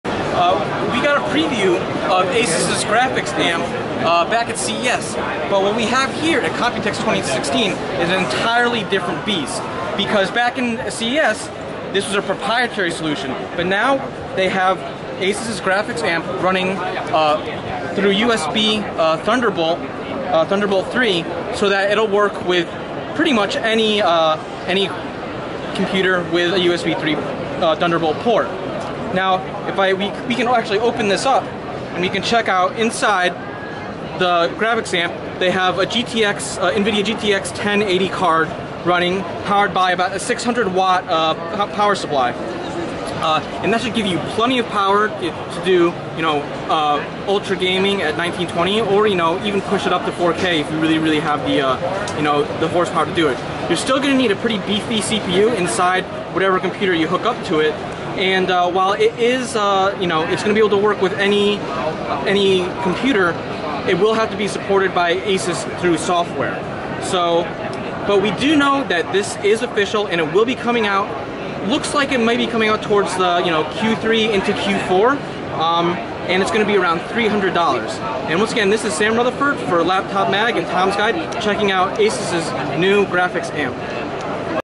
Asus's New Graphics Amp Hands-On
For under $300, this amp will connect to any Thunderbolt 3 laptop and help it play real VR games, provided you bring your own video card.